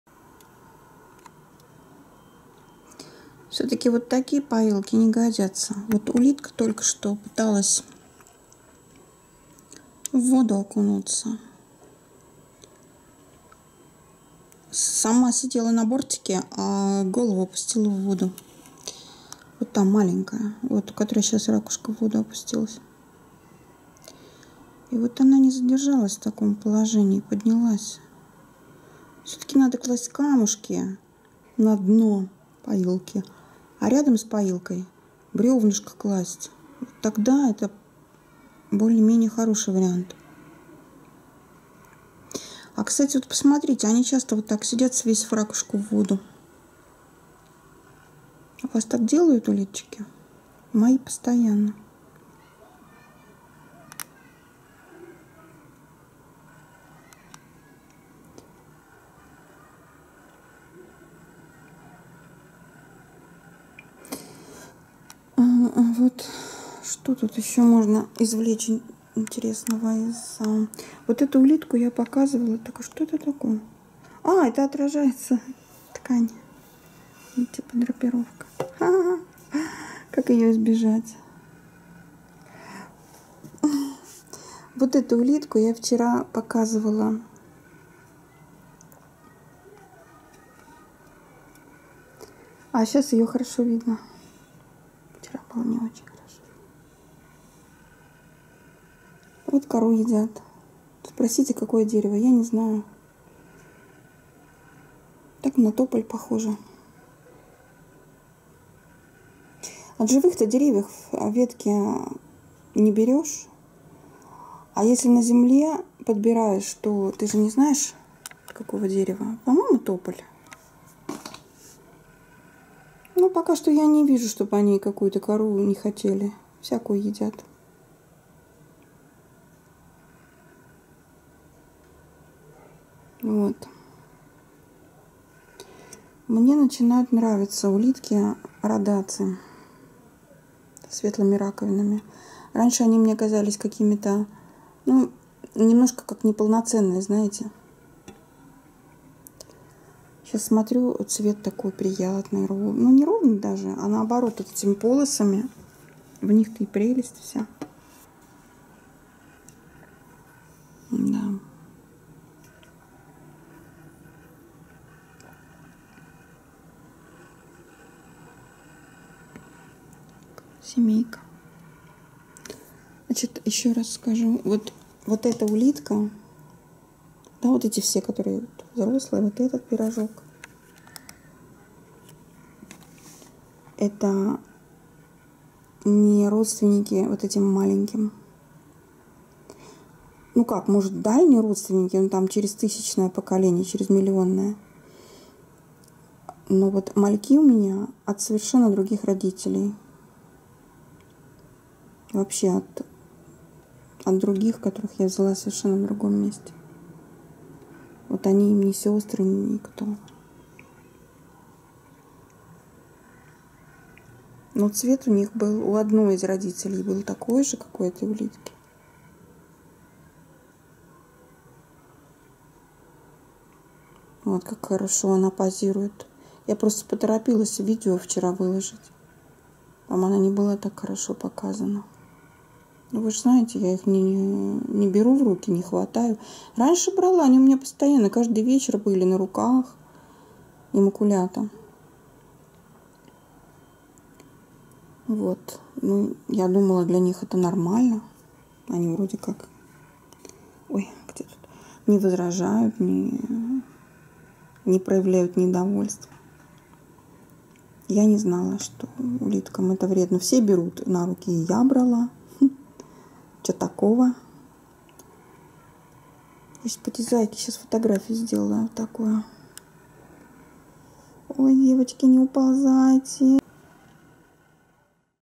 Ахатина фулика родаци.
Фулики со светлыми раковинами всё больше привлекают моё внимание.